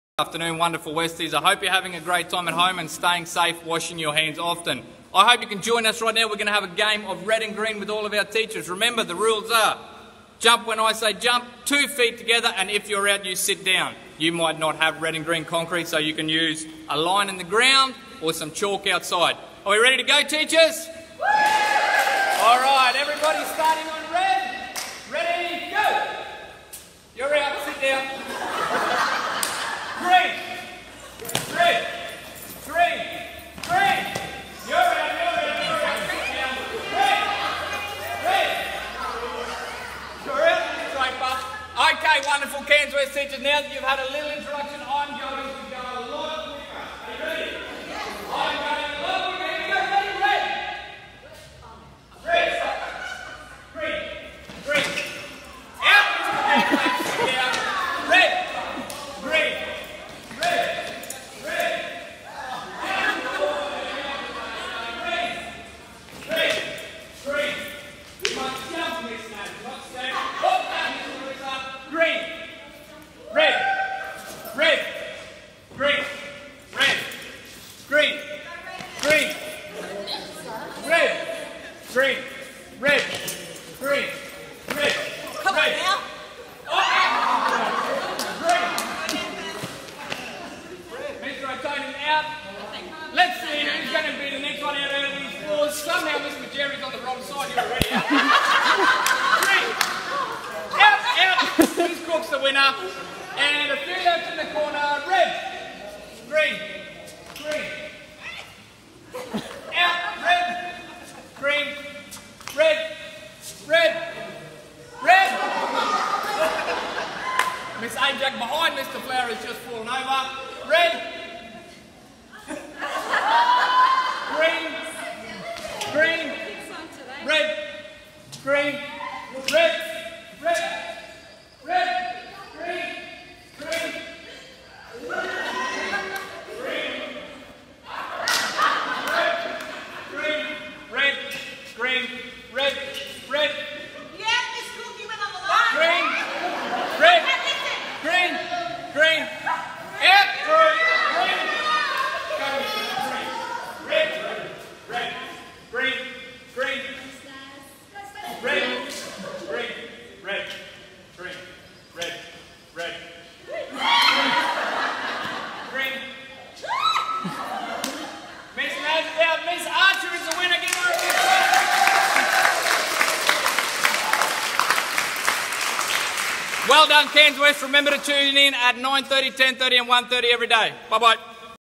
Cairns West Staff - Red/Green Game
When the kids are away, the staff will play...

“Our school is a community in which people are valued and our students are given the opportunity to develop in learning, responsibility and character."

School motto: “Learn now to succeed”